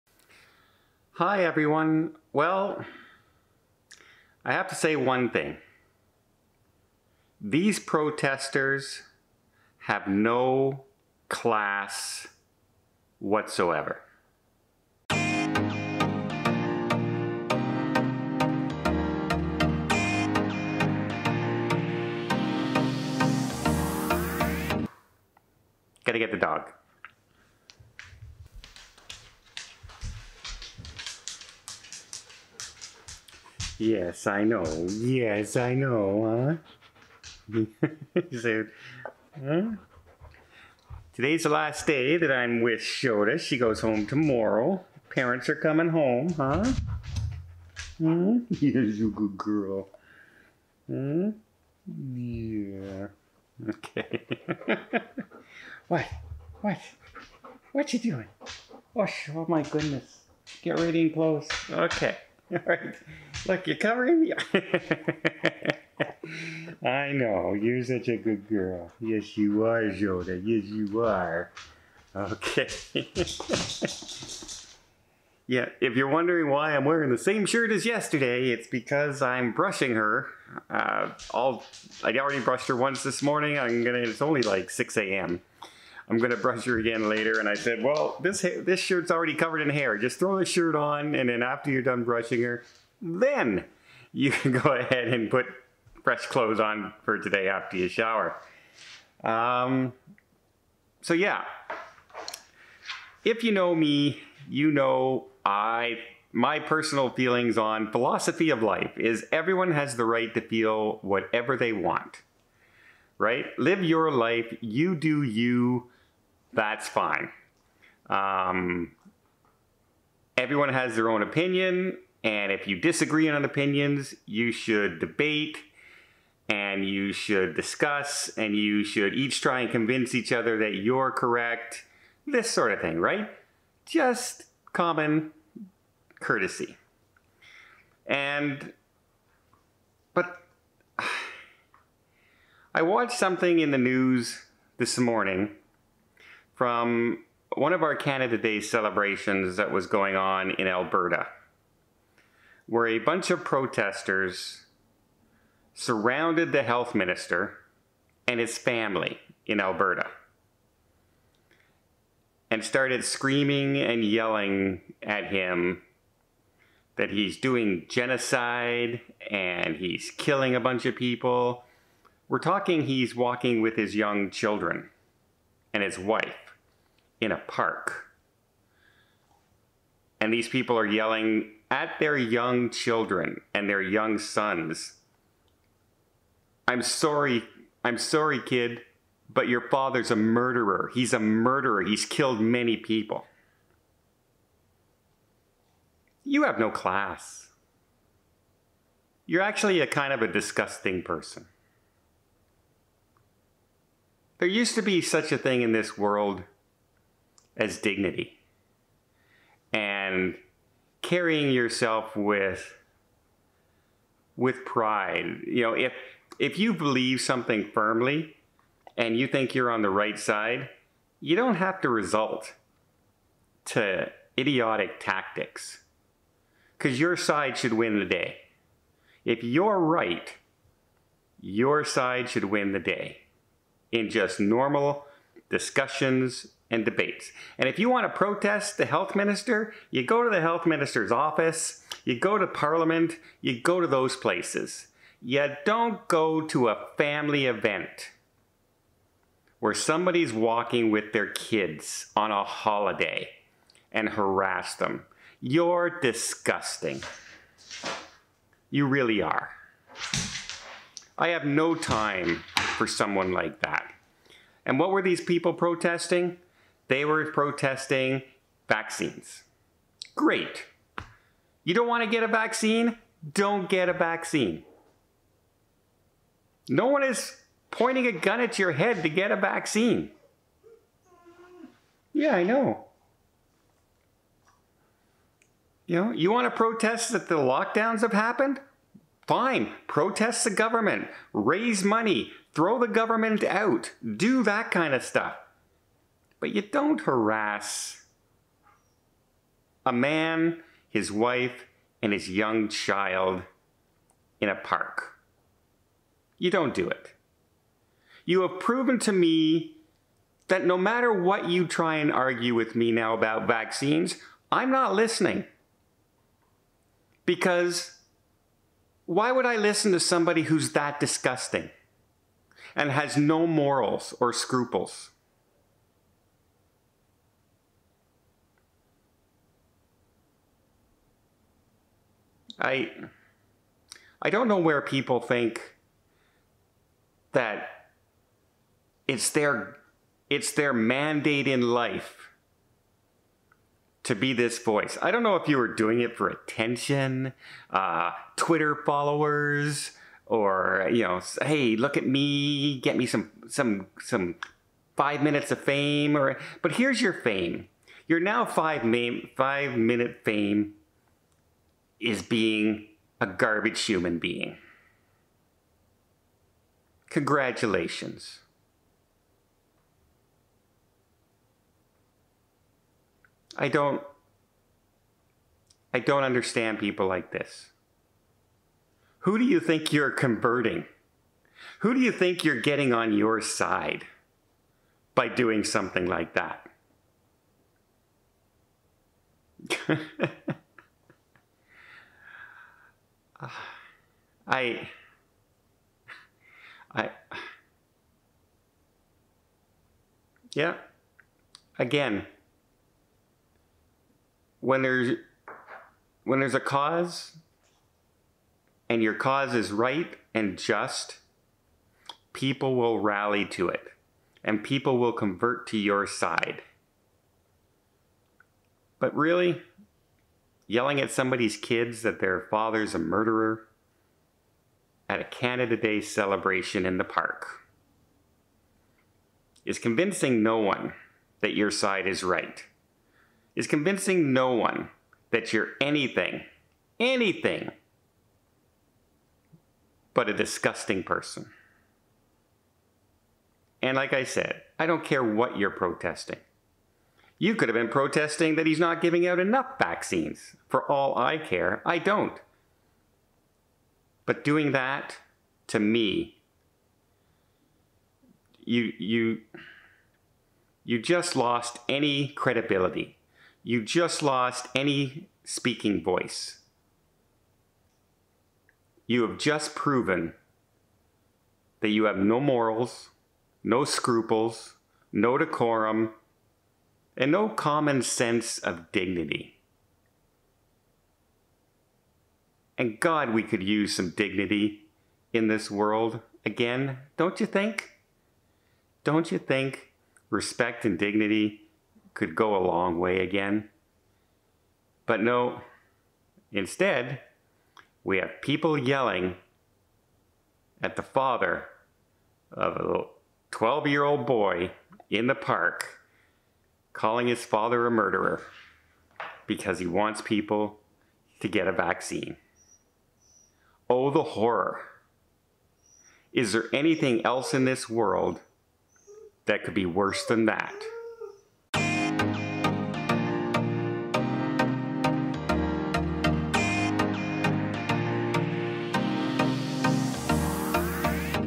This Protester Disgusts Me - Vlog 179
This Protester Disgusts Me. You have the right to protest in this world.  However this is a time and place.  Also attacking someone's family is a completely trash move.
My Mailing address is:
Don's Family Vacations PMB #471​​​​​​​​​​​​​​​​​​​​​​​​​​​    900 Greenbank road Nepean Ontario K2J 4P6